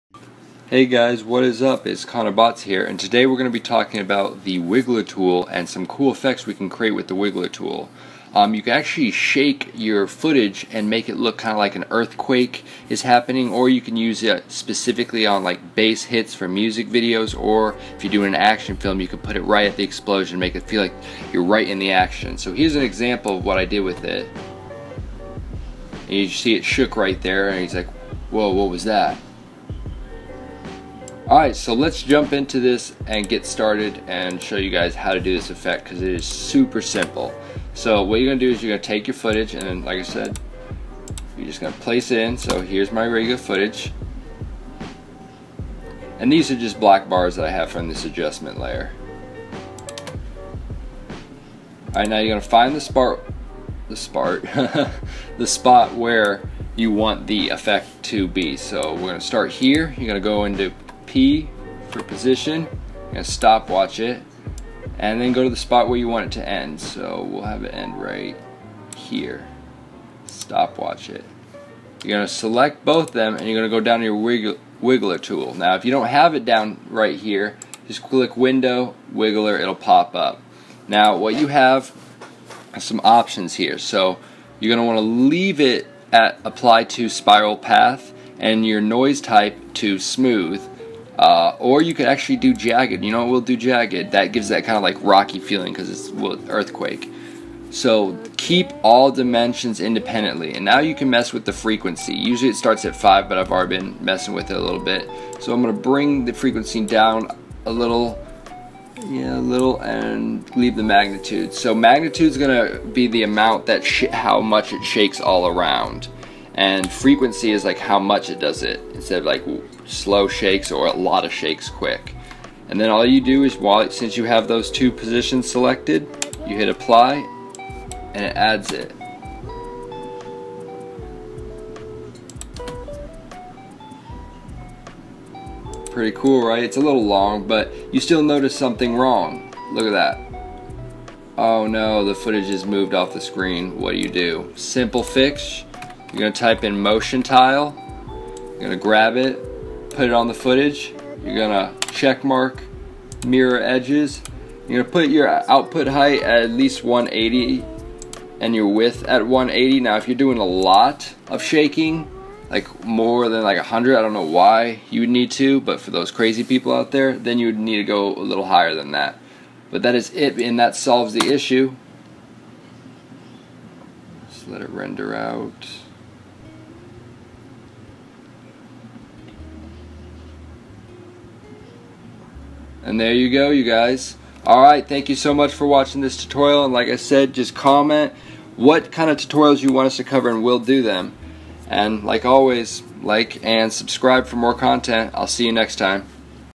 How to Create Fake Camera Shake | After Effects Tutorial
In this tutorial we show you how to create fake camera shake for your next project, quick and fast!